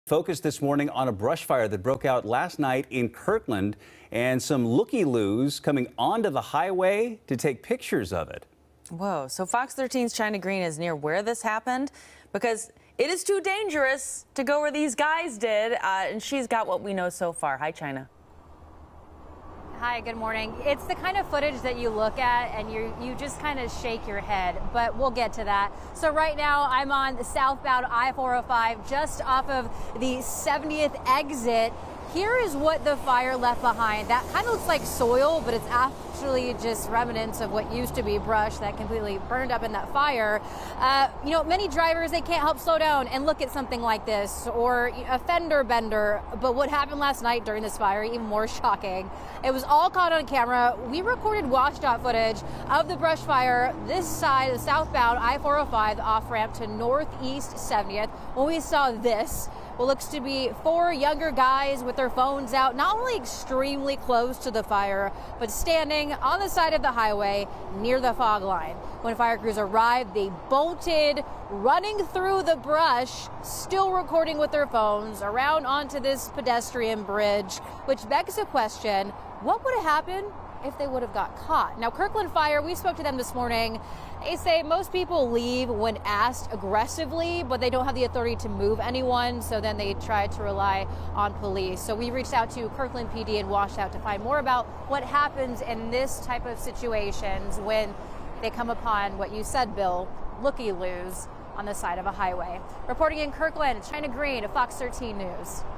Group spotted on off-ramp recording brush fire in Kirkland
A group of what appeared to be young men were caught on WSDOT's cameras on an on-ramp recording a brush fire in Kirkland.